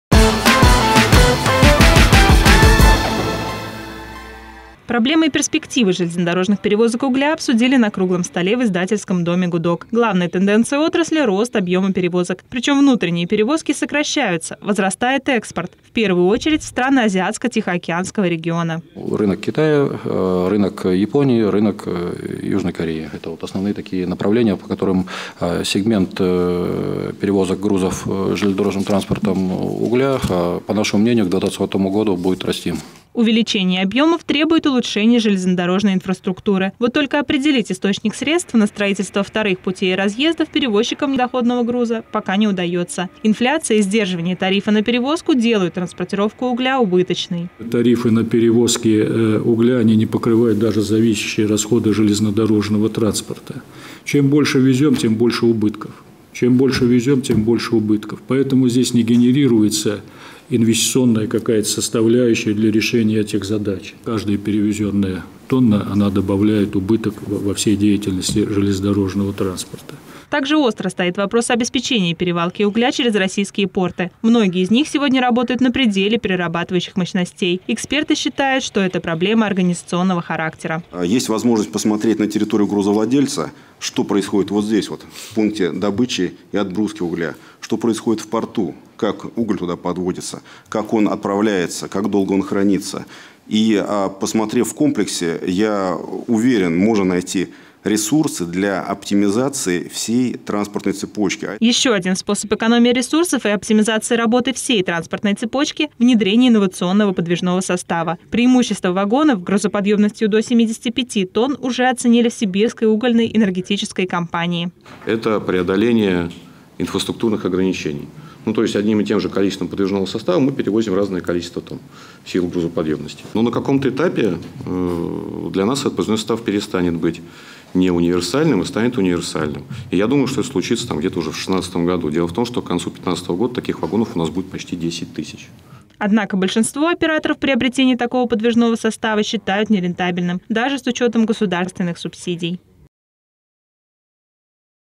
Перевозка угля стала убыточной
Проблемы и перспективы железнодорожных перевозок угля обсудили на круглом столе в Издательском доме «Гудок»